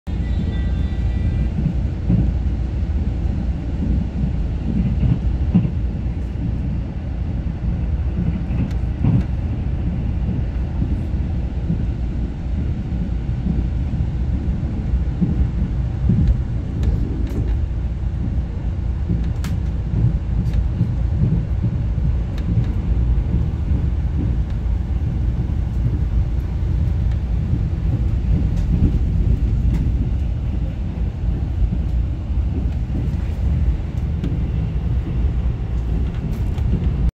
[4K 60fps 10x광학줌] 20230906 134020 SRT가 지나갈 때가 되었는데.. 무궁화1503 탑승중
정해진 시간표 대로 딱딱 지나가는 적이 없음..

촬영 : Samsung Galaxy S 22 Ultra (SM-S908N)